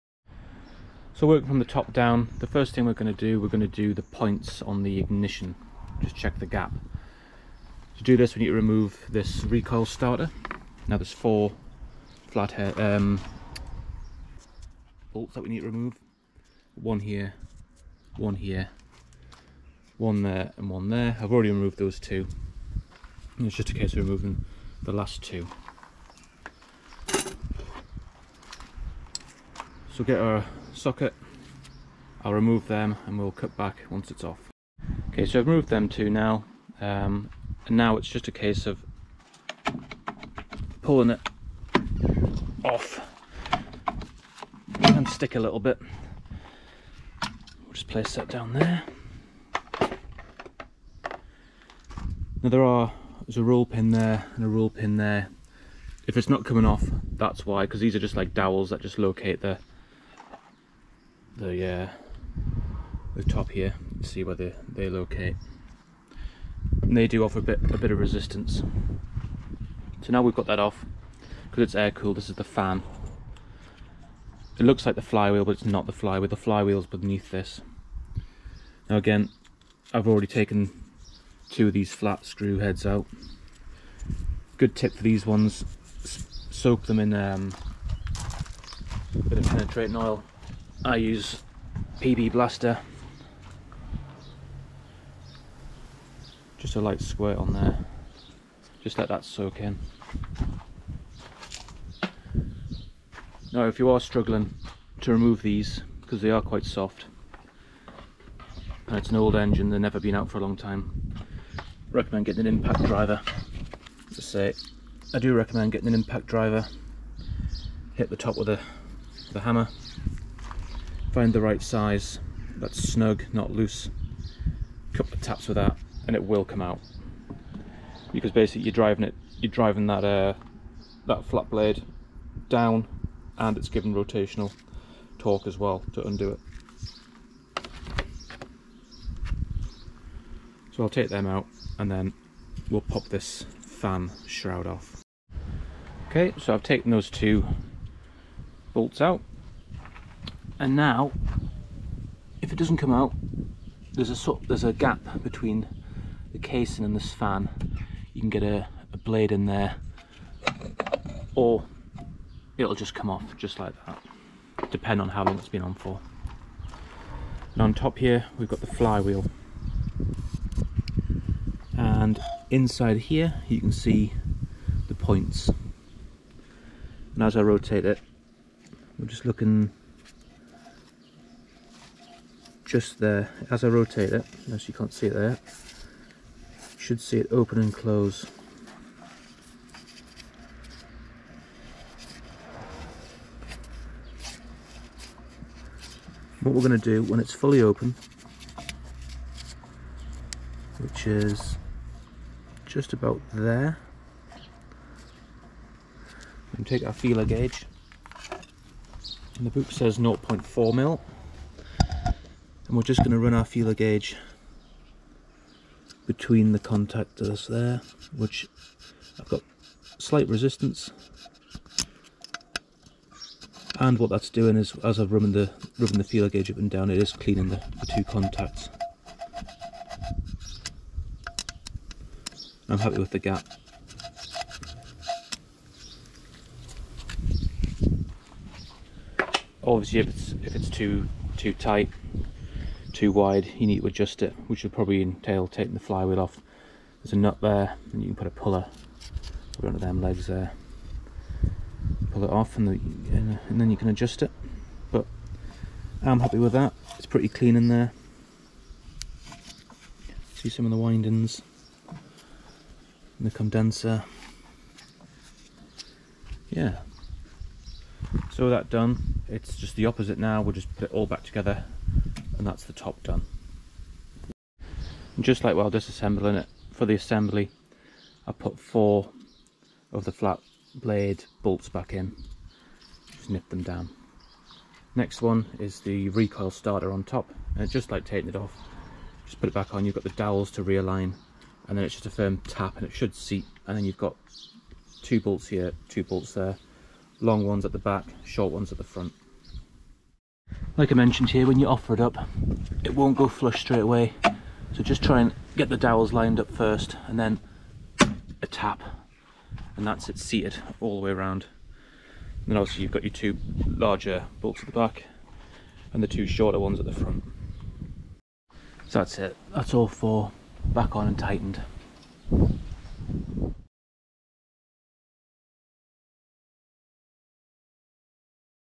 How To Check - Ignition Points Gap - Tomos T4 4hp Ailsa Craig Outboard Engine
In this video, we have a look at how to check the ignition points gap on a Tomos T4 4hp Ailsa Craig Outboard engine.

We go into how to remove the recoil starter, fan, and then check the points gap which on this engine 0.4mm ( 0.35mm - 0.45mm )